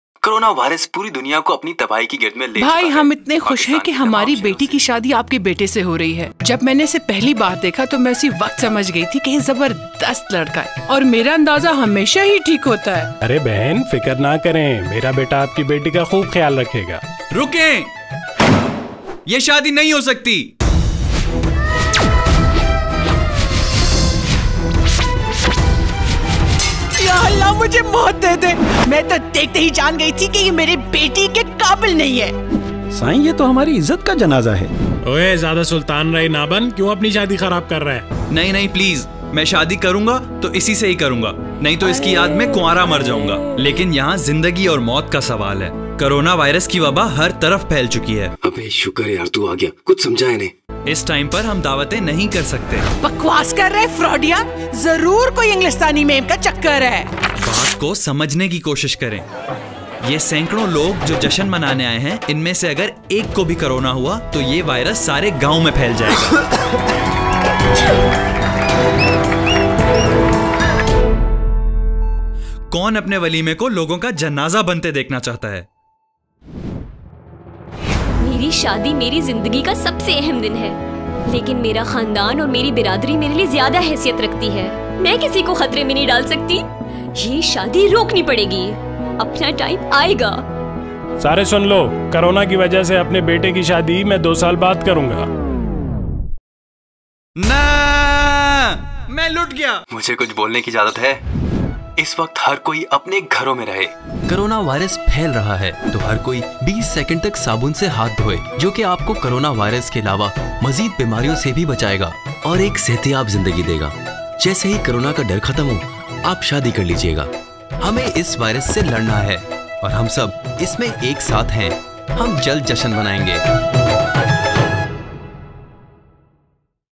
The Wedding ( Urdu ) | Corona Virus Awareness Video | 2020 | Studio Rokhan
Here is our latest project that focuses on creating awareness on Corona Virus - on the need to withhold any kind of events during this time.

Directed / Animated by: Zeeshan Parwez
Concept: Fasi Zaka
Music Producer: Sarmad Ghafoor
Voice-Over Production: Saad Hayat Music, Sarmad Ghafoor
Voice-Over Artists:  Shandana Sarmad, Syeda Natalia Gul, Ali Sangi, Abbas Haider Sangi, Zulfiqar Ali Shah, Saad Hayat
Environment Illustrations: Samad Rizvi
Scripts:: Fasi Zaka, Shawana Zeeshan Parwez